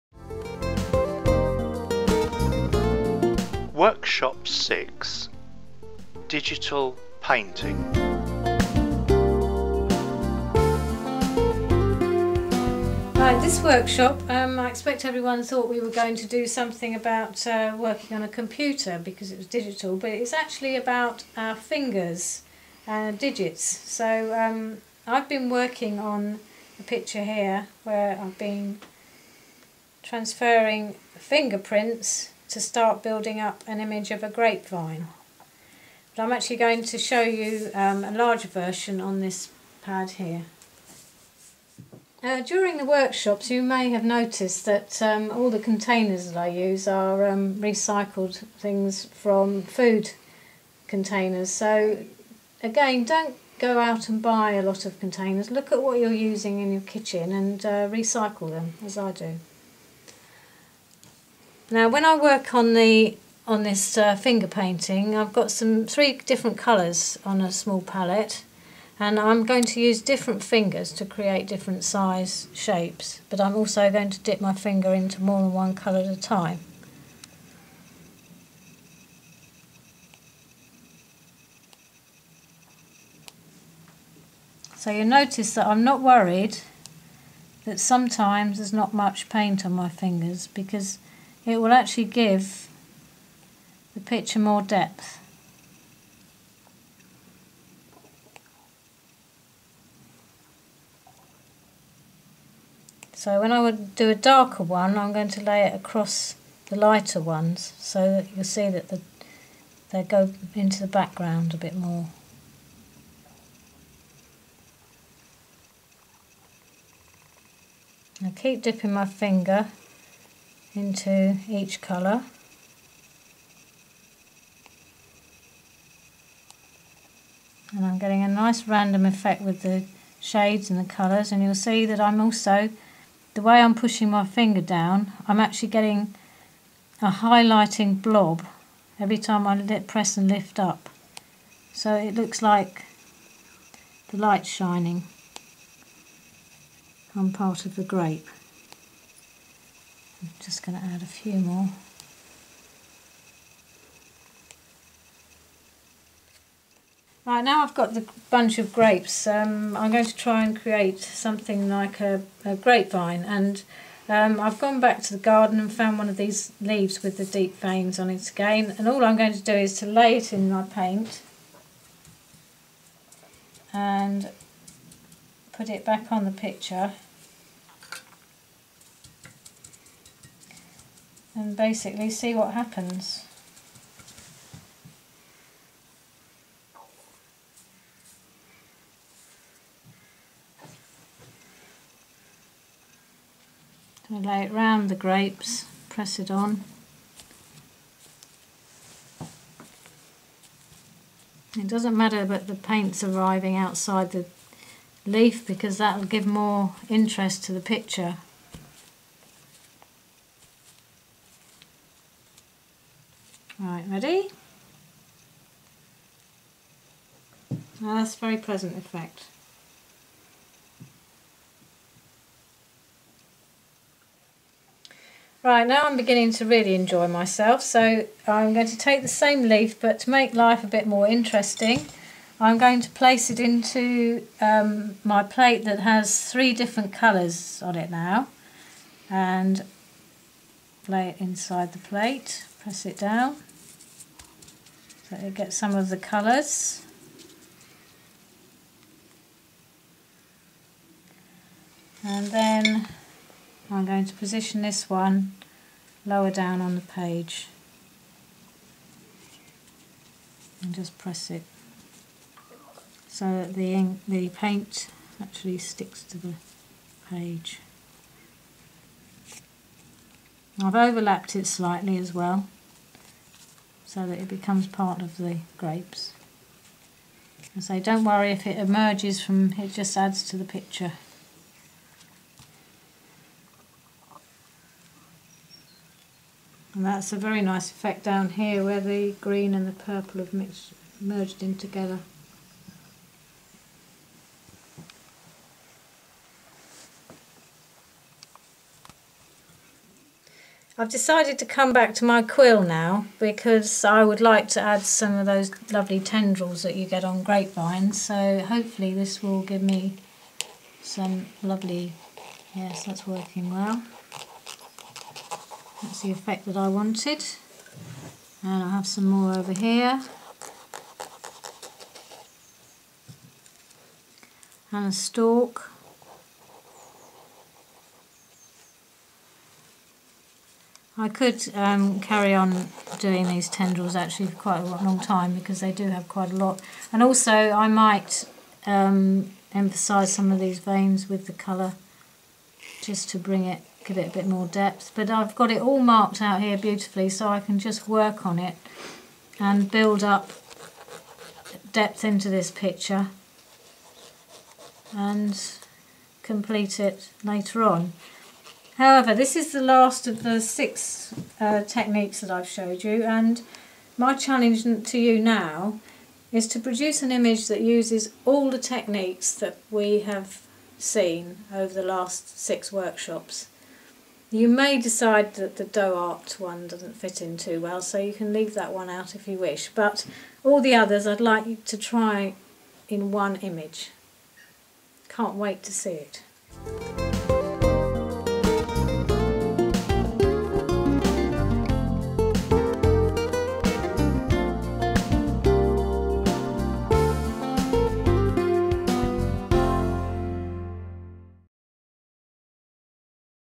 Willow Workshop No 6 - "Digit"al Painting
This is the 6th and final workshop by Annie Begley and show how to draw and paint by finger.